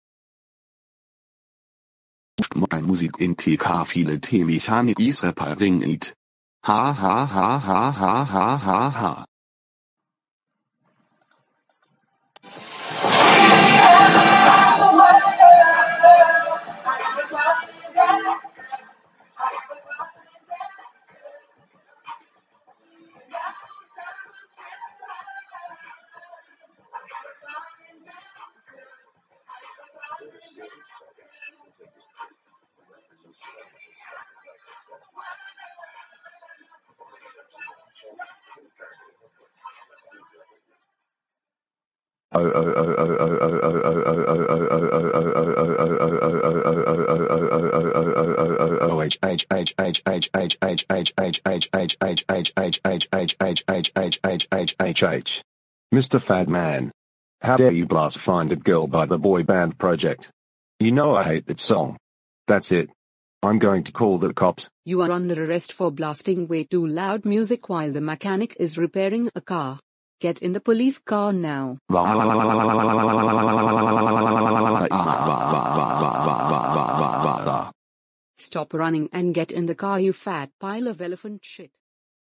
Mr. Fat Man interrupts a Mechanic and gets arrested.
He interrupted a Mechanic by blasting loud music!